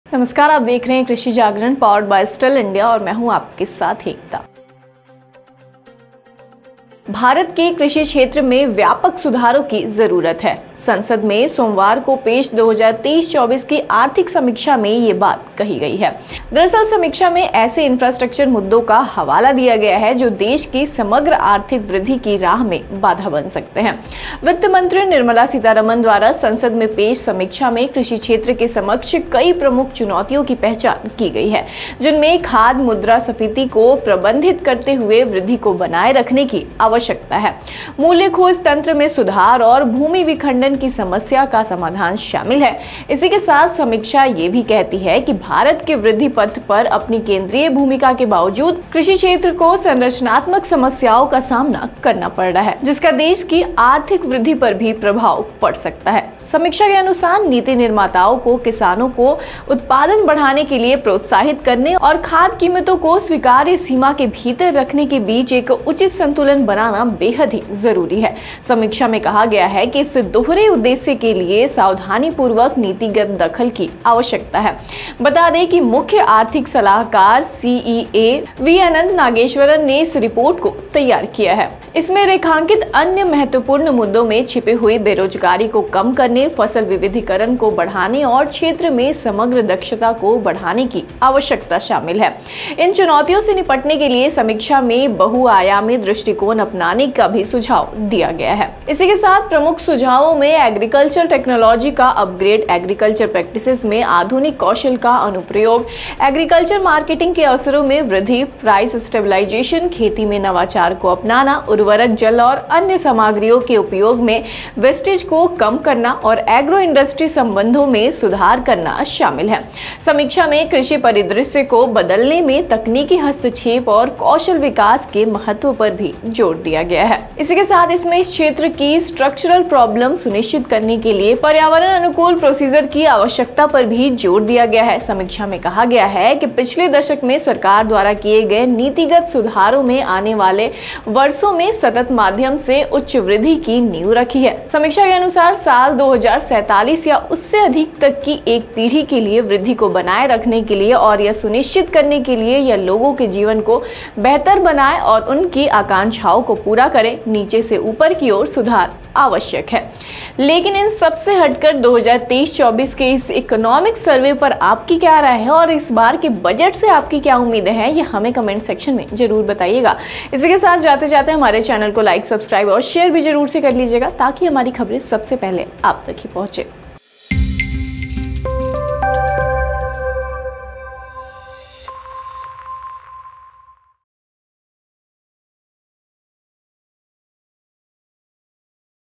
Economic Survey 2024: सर्वे में कृषि सुधारों को आगे बढ़ाने पर जोर, तकनीक और स्किल डेवलपमेंट पर फोकस
Economic Survey 2024: सर्वे में कृषि सुधारों को आगे बढ़ाने पर जोर, तकनीक और स्किल डेवलपमेंट पर फोकस | Budget 2024 | Nirmala Sitharaman | PM Modi |

Economic Survey 2024: वित्त मंत्री (Finance Minister) निर्मला सीतारमण ने संसद में आर्थिक समीक्षा 2023-24 पेश की. भारत के कृषि क्षेत्र में व्यापक सुधारों की जरूरत है. संसद में पेश 2023-24 की आर्थिक समीक्षा में यह बात कही गई है. समीक्षा में ऐसे संरचनात्मक मुद्दों का हवाला दिया गया है, जो देश की समग्र इकोनॉमिक ग्रोथ(Economic Growth) की राह में बाधा बन सकते हैं.

Script- Ekta Tripathi
Anchor- Ekta Tripathi
Camera Person- Aashish
Video Editor- Bhavna